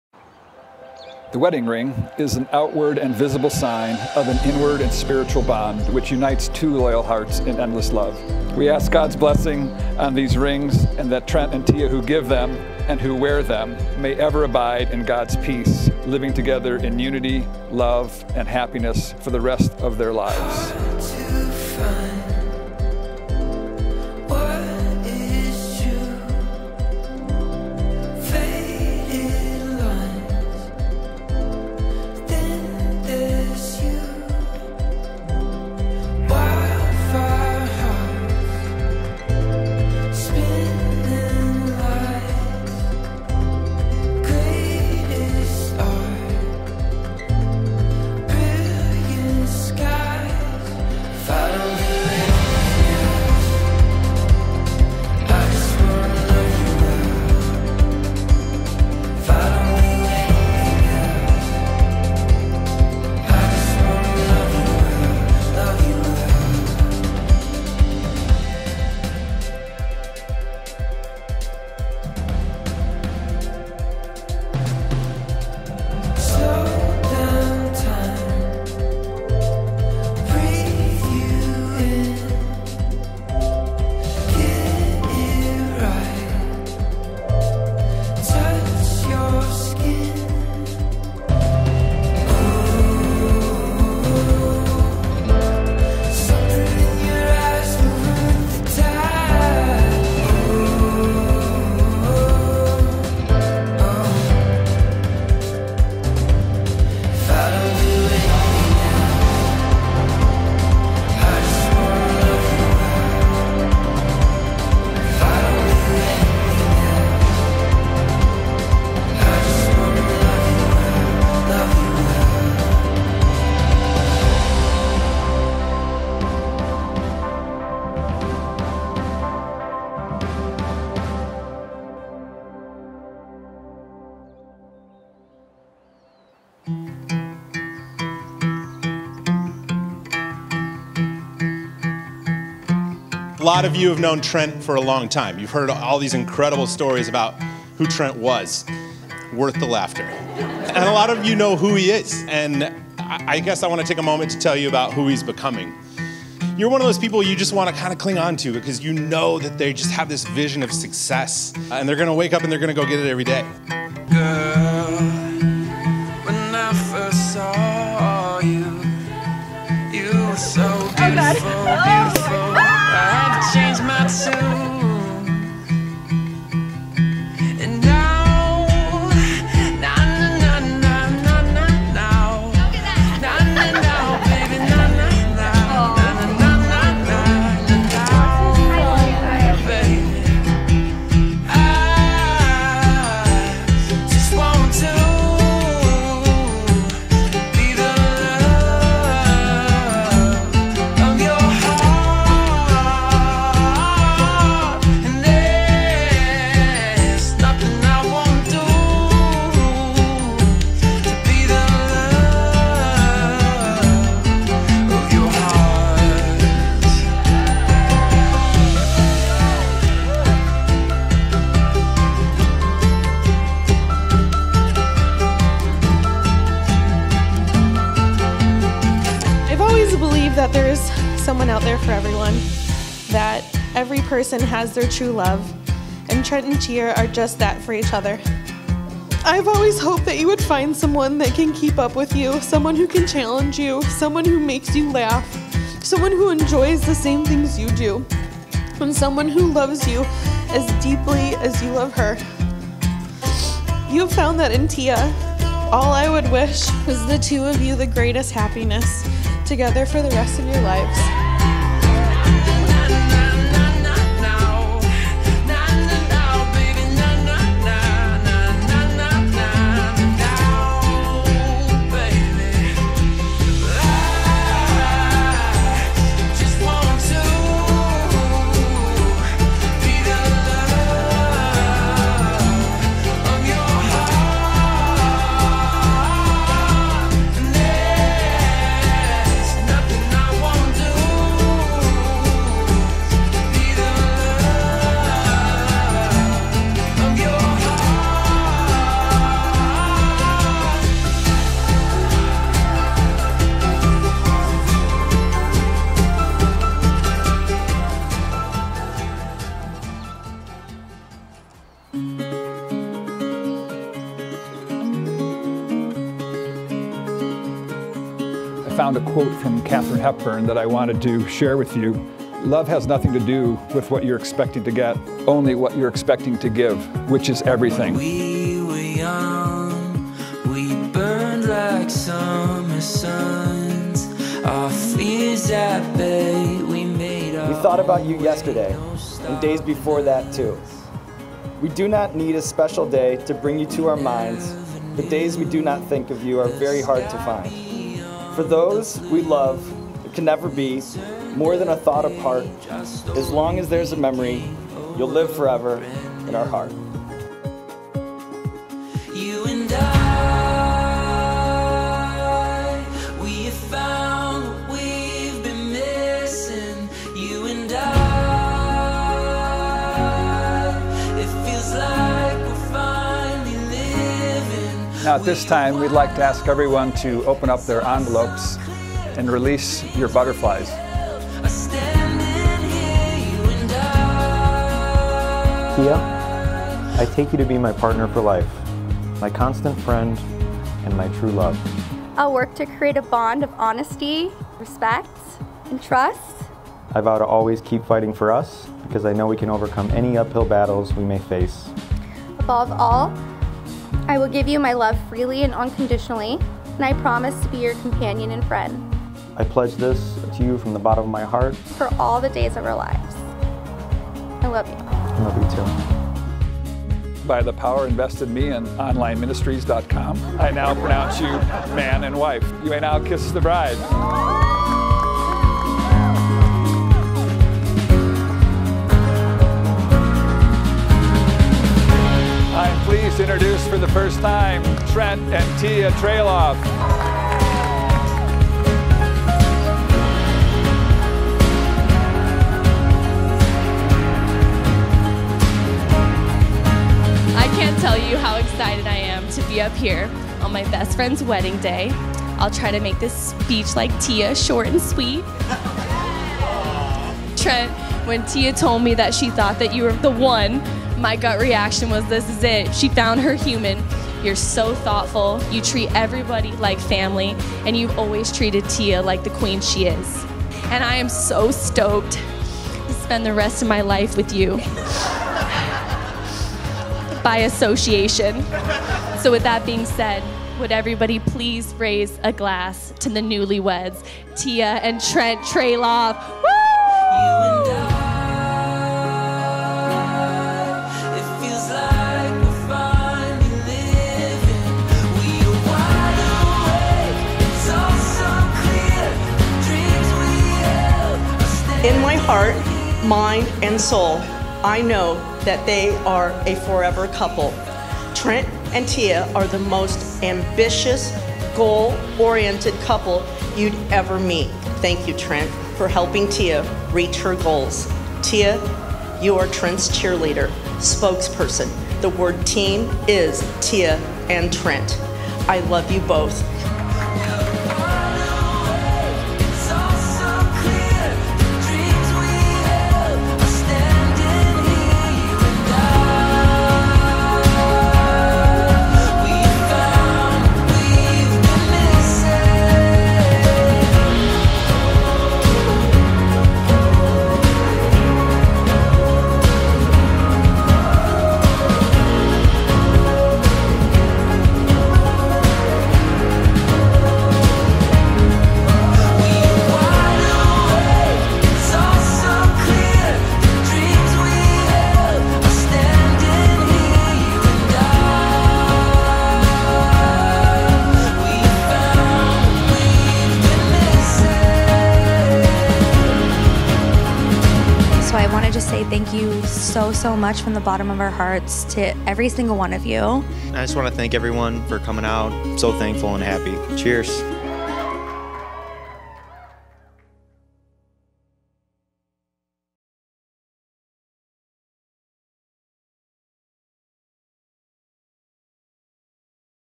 El Chorro Wedding Video • Trent + Tia • Sedona & Paradise Valley, Arizona
Wedding Date: December 11, 2020
Venue/Location: El Chorro in Paradise Valley, AZ

This was the first time we've had the opportunity of capturing a pre-wedding video shoot in beautiful red rock Sedona, AZ. We took the 90-minute drive from Phoenix a week before the wedding. What are your thoughts on including pre-wedding footage in a wedding film?

All music licensed from the Music Bed
MB01T2TYPBIMTQA